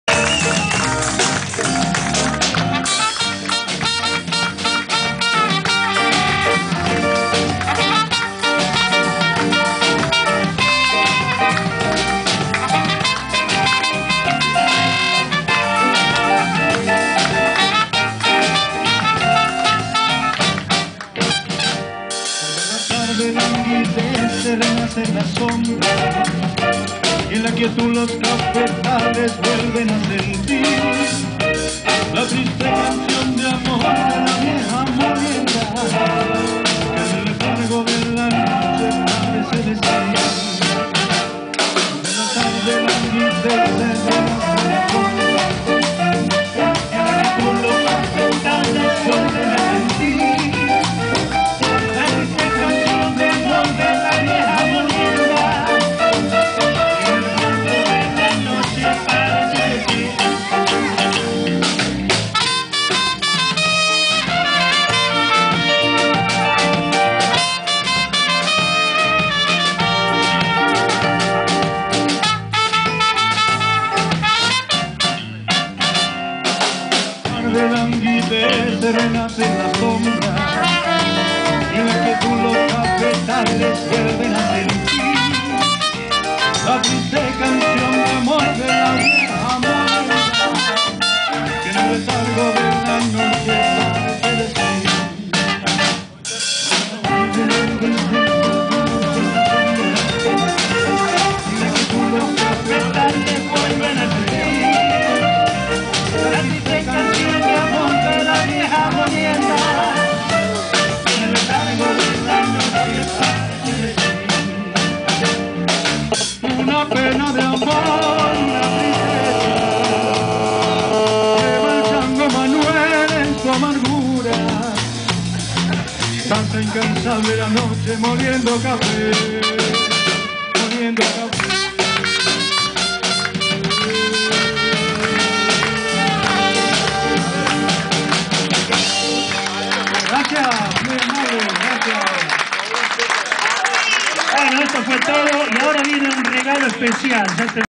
melody swing6 2009
como todos los años, el homenaje a los medicos por melody swing..